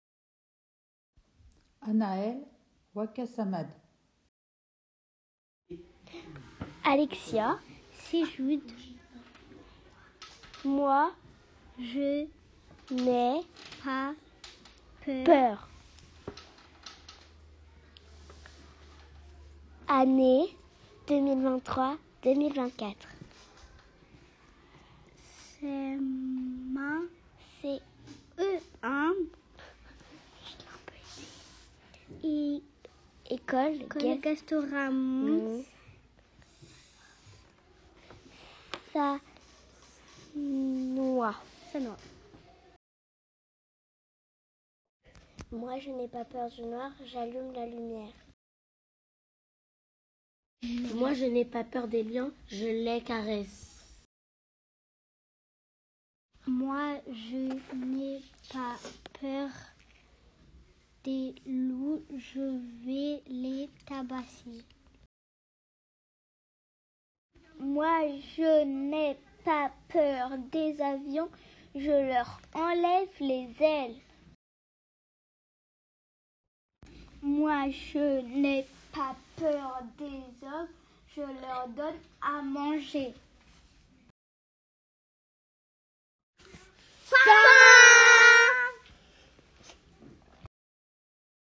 Je n’ai pas peur
A partir d'un album des éditions PEMF, les élèves de CE1 ont inventé une suite...